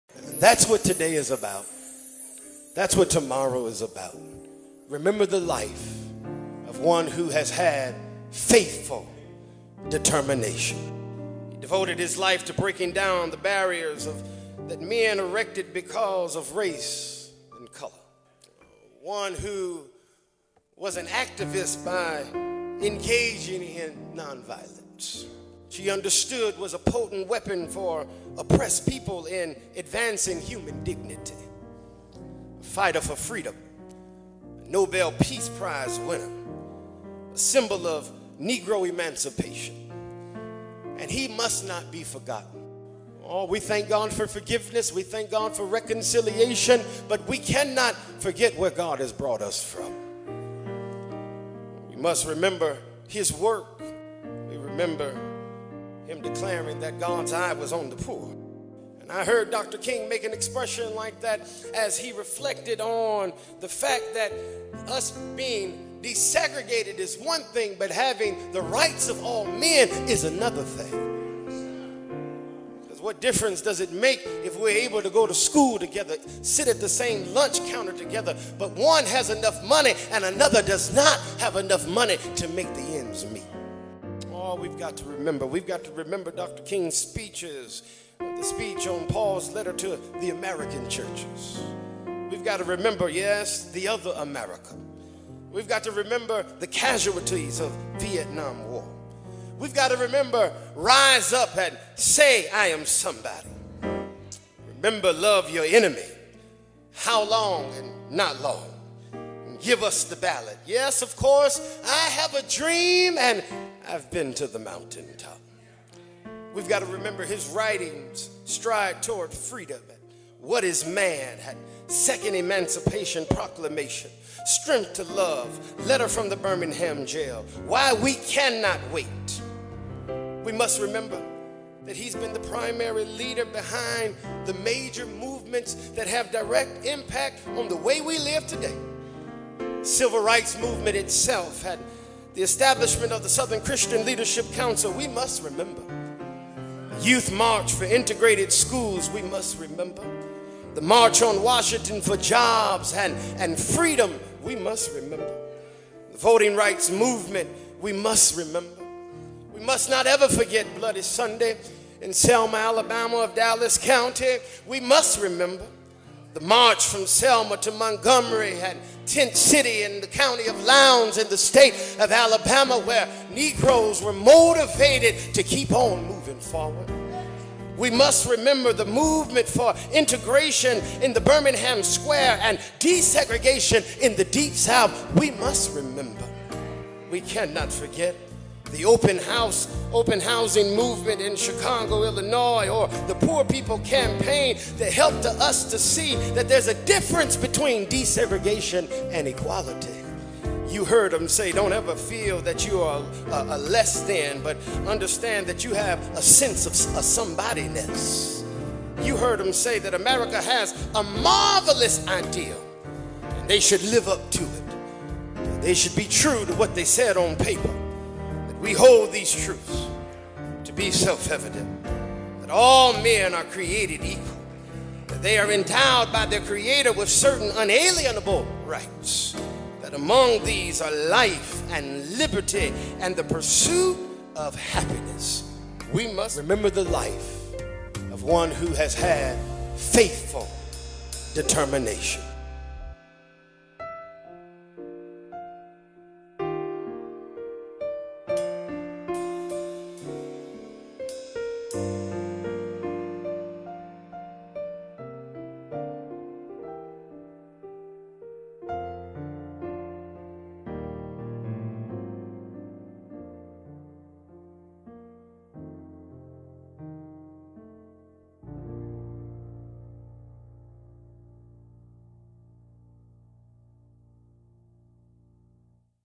Tribute to Dr. Martin Luther King Jr.
Let's pause for a moment as we give tribute to Dr. Martin Luther King, Jr.